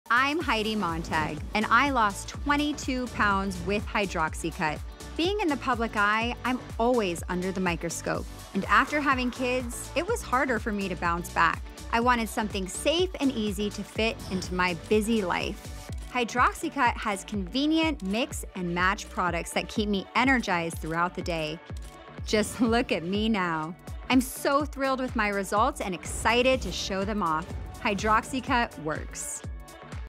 Heidi Montag for Hydroxycut
As part of her search for the perfect weight loss supplement, Heidi Montag wanted something safe and easy to fit within her busy life. Hydroxycut has convenient mix and match products that keep her energized throughout the day.

In short, Hydroxycut works. Heidi is proof of it!